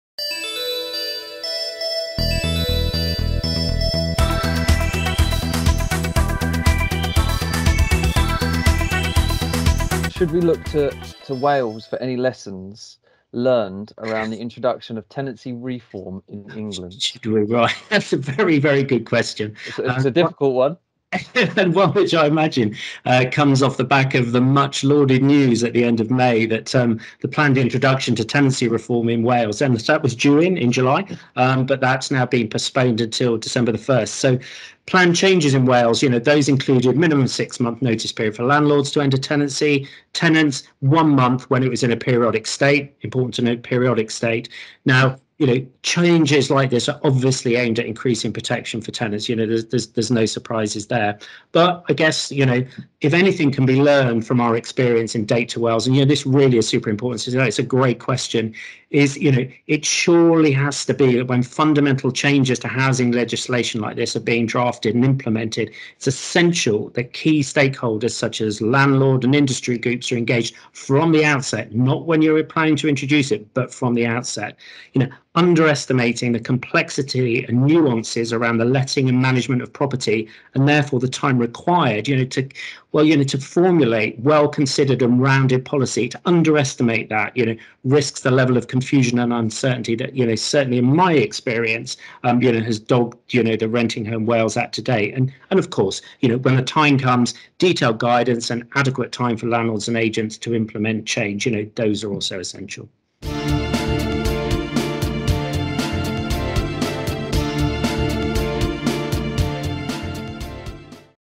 Interview with Andrew Culverwell on Learning From Wales
In this video, Lee Dahill of Angels Media speaks with Andrew Culverwell  from Connells Group to discuss tenancy reform and whether we should look to Wales for any lessons.